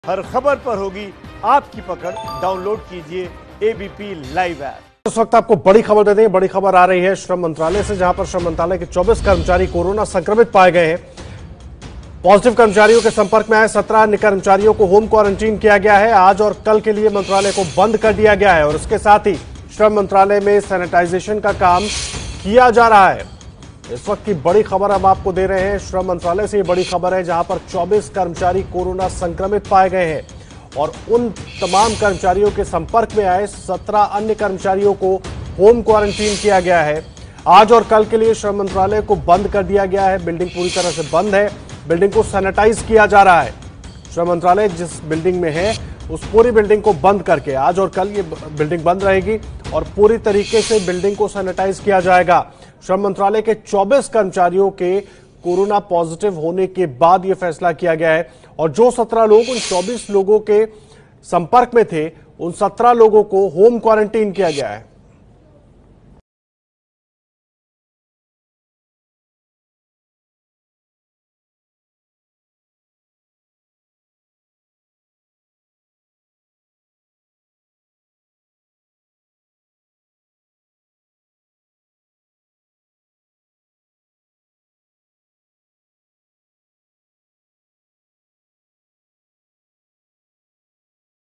श्रम मंत्रालय के 24 कर्मचारी Corona पॉजिटिव, संपर्क में आए 17 लोग होम क्वारंटीन | ABP News Hindi
श्रम मंत्रालय के 24 कर्मचारी Corona पॉजिटिव पाए गए हैं. इनके संपर्क में आए 17 लोग होम क्वारंटीन किए गए हैं. मंत्रालय दो दिनों के लिए बंद कर दिया गया है.